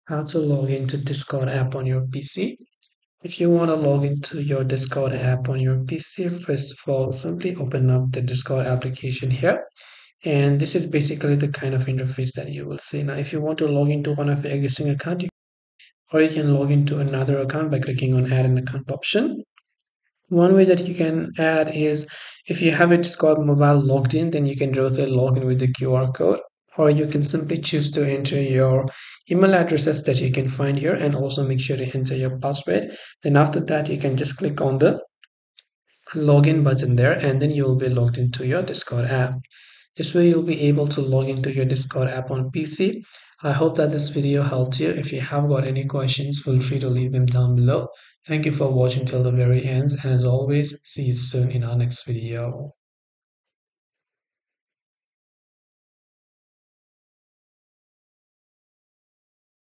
How to Login Discord App on PC
This video demonstrates how to log into your Discord account on your PC. The tutorial covers two main methods:

QR Code Login: If you're logged into Discord on your mobile device, you can scan a QR code on the PC screen to seamlessly log in.

The video also mentions the option to add another account, presumably for users who manage multiple Discord accounts.

Chapters:
0:00 Introduction
0:14 Opening Discord App on PC
0:16 QR Code Login
0:28 Email and Password Login
0:41 Conclusion